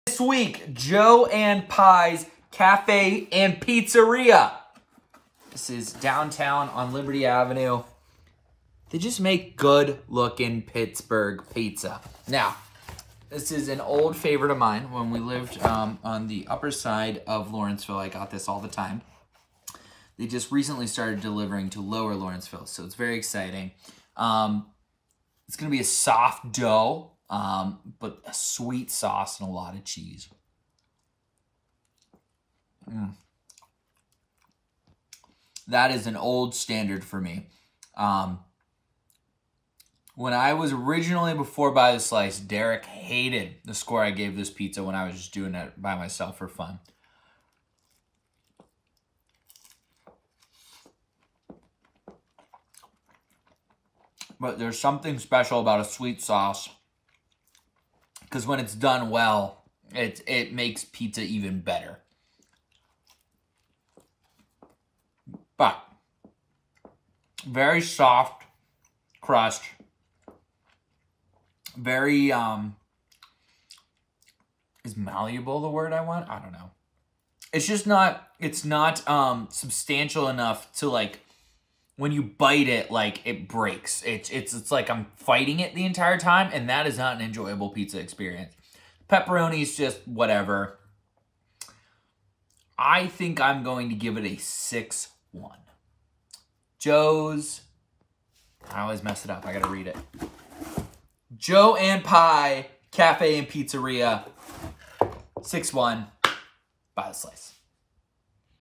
By The Slice (Joe and Pie Pizzeria & Cafe)
This week Josh Elsass dives into the downtown pizza scene with Joe&Pie Cafe Pizzeria